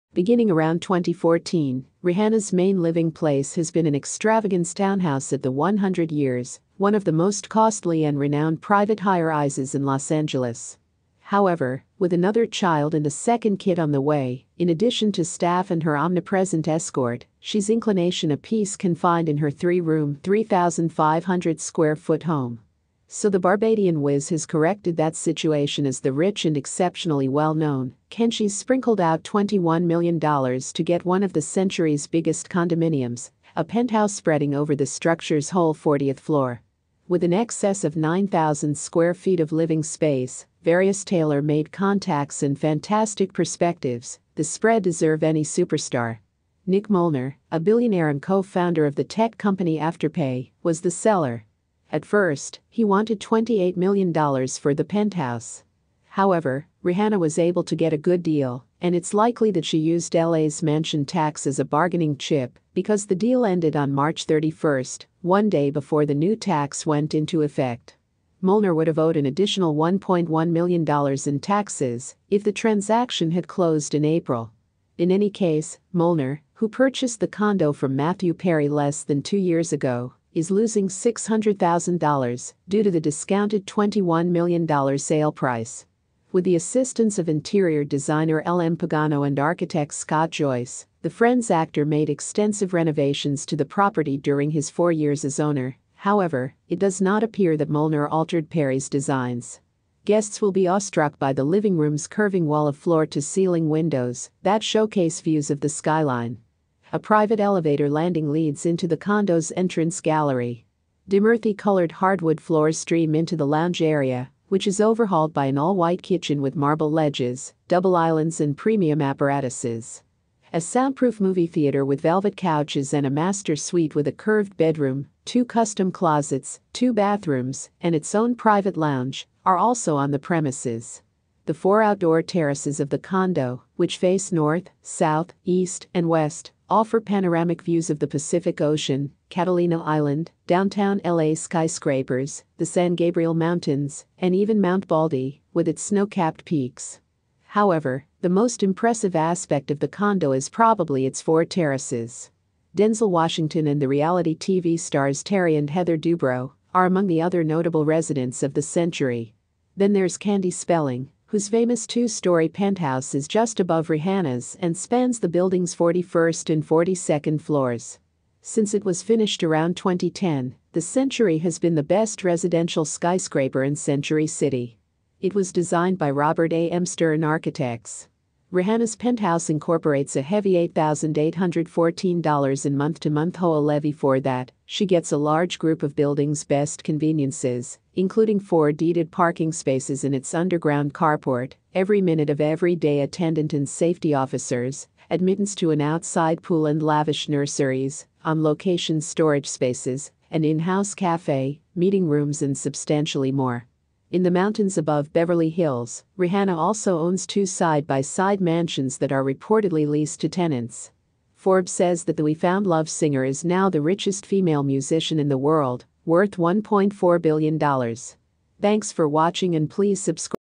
Details of Rihanna Penthouse Bought at $21 Million Los Angeles/Big house, Big Family
Billionaire singer and business woman buys a penthouse for her family#rihanna